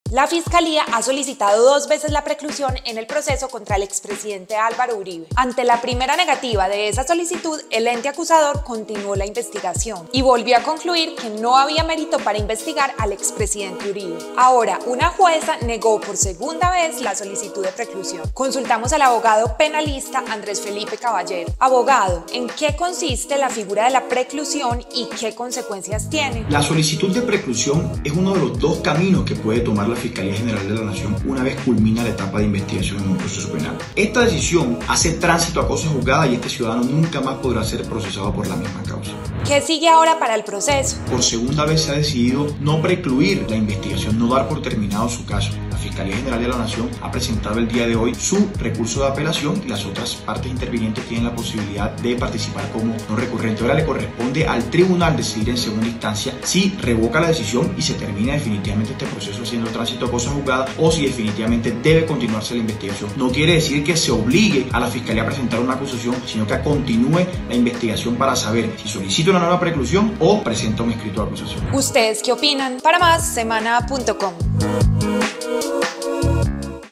¿Qué sigue en el proceso judicial contra Álvaro Uribe Vélez?
Julia Correa habló con un abogado para conocer cómo está el proceso del expresidente Álvaro Uribe Vélez, luego de que una juez negara por segunda vez la solicitud de preclusión de la Fiscalía.